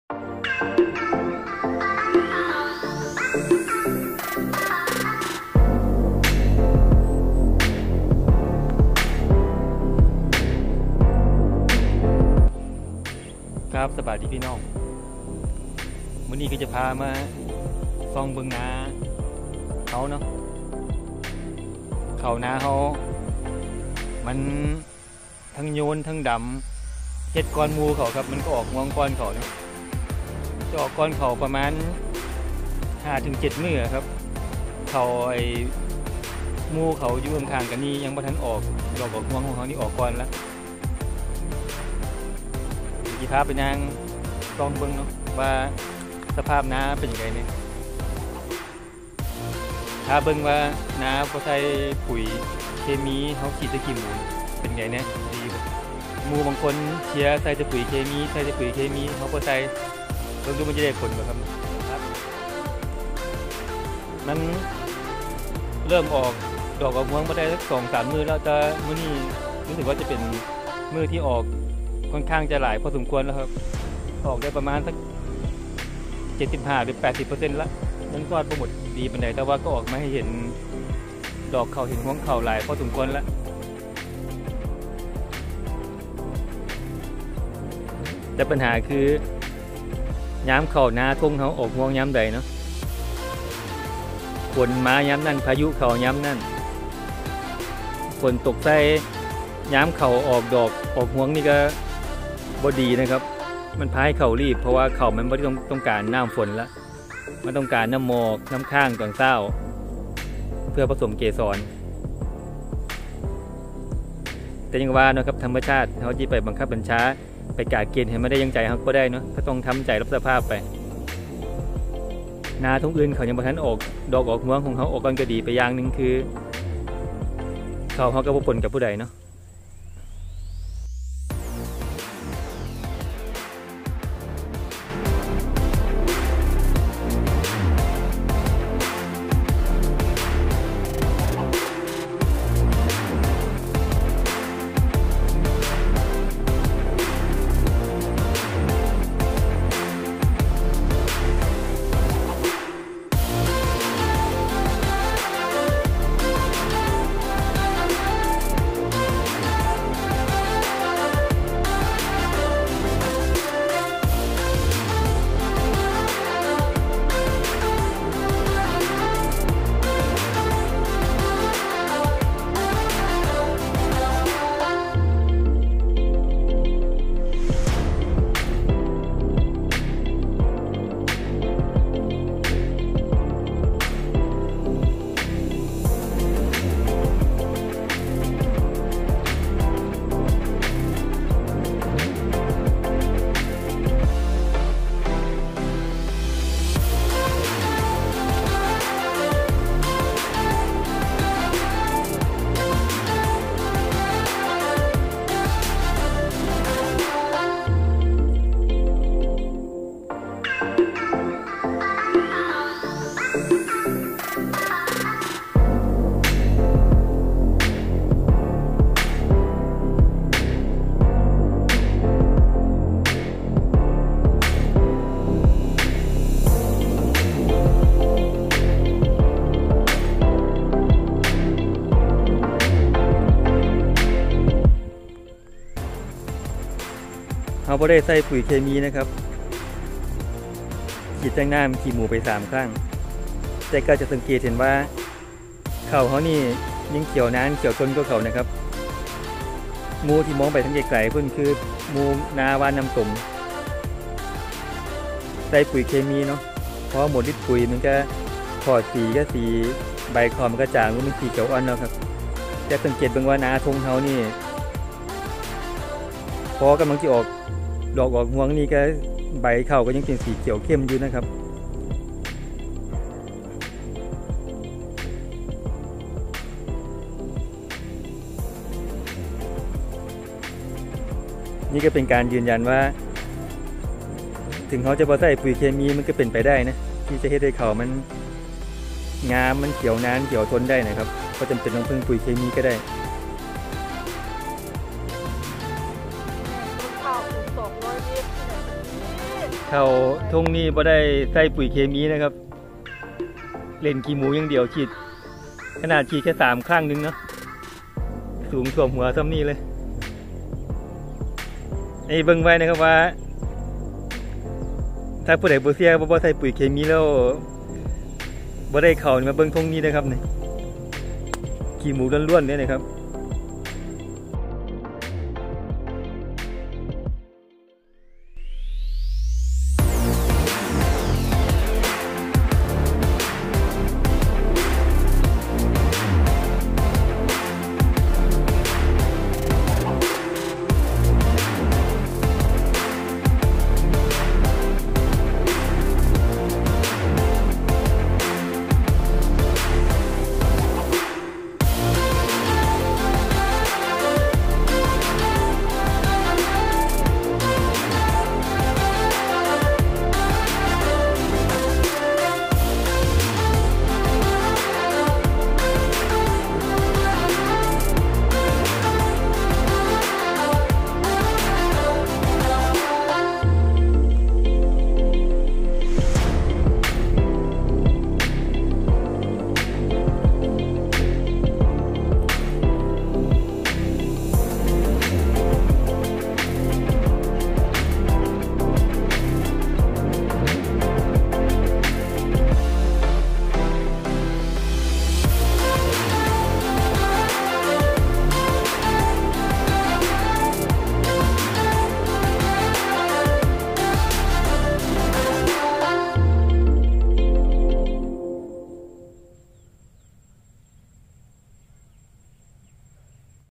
ผลงานการใช้น้ำขี้หมูฉีดพ่นในนาข้าว ข้าวระยะออกรวง ข้าวสวยใสไร้ปุ๋ยเคมี
ลดต้นทุนการใช้ปุ๋ยเคมีโดยการใช้น้ำขี้หมูฉีดพ่นในนาข้าวทุกๆสองอาทิตย์ ผลงานหลังจากฉีดน้ำขี้หมูในนาไปได้สามครั้ง ข้าวเข้าสู่ระยะเริ่มออกดอกออกรวง ข้าวขาวดอกมะลิ 105